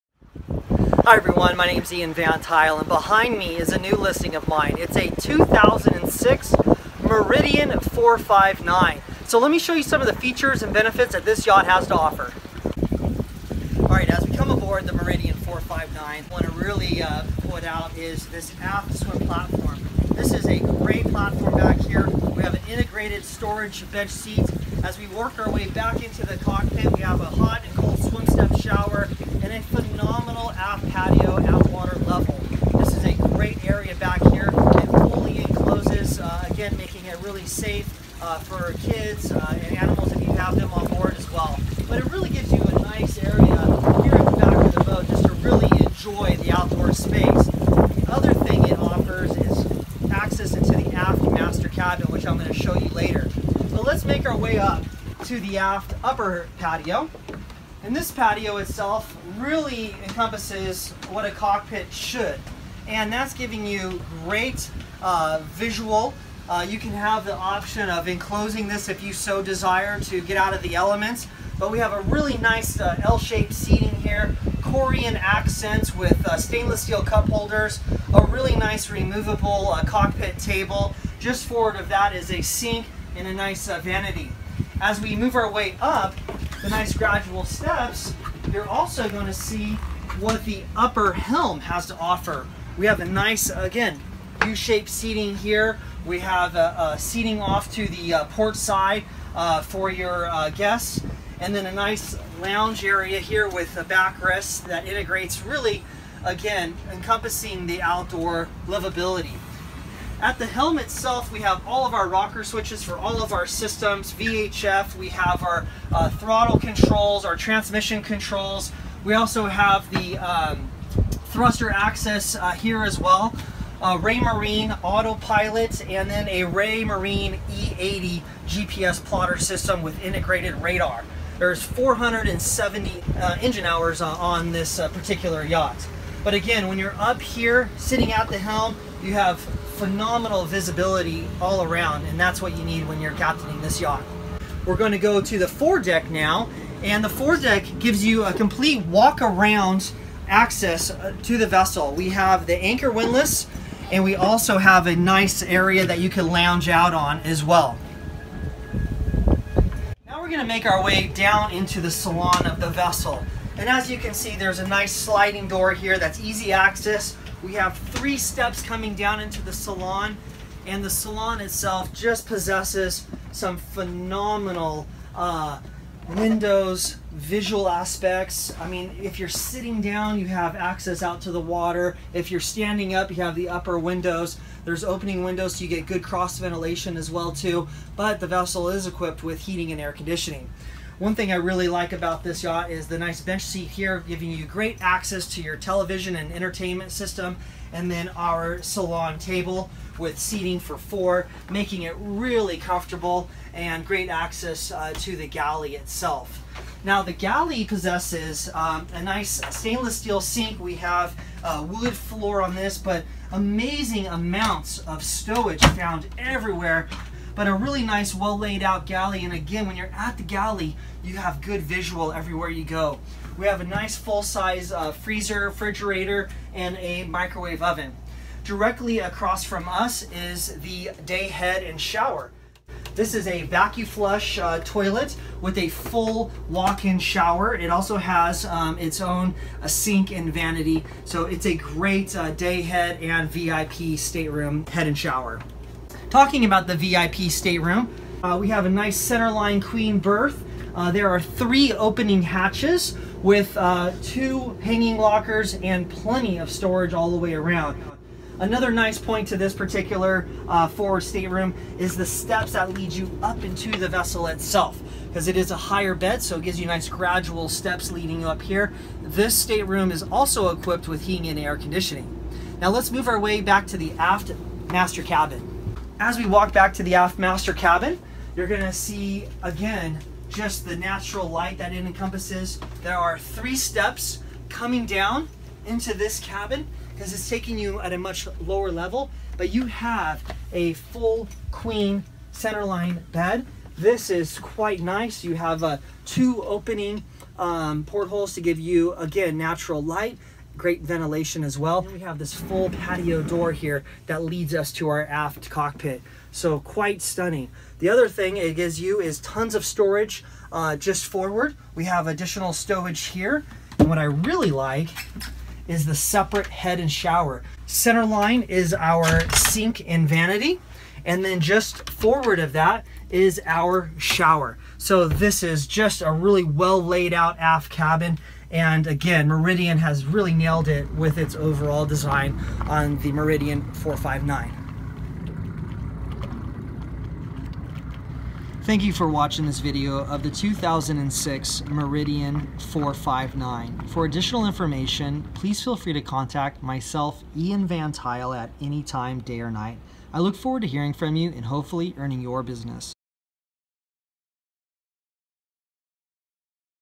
New Motor Yacht Listing! 2006 Meridian 459 Walk Through By: Ian Van Tuyl
2006 Meridian 459 Motor Yacht

For additional information on this 2006 Meridian 459 Motor Yacht please contact Ian Van Tuyl the Yachts specialist.

Google +  - IVT Yacht Sales, Inc.
Facebook - IVT Yacht Sales, Inc.
Blogger    - ivtyachtsales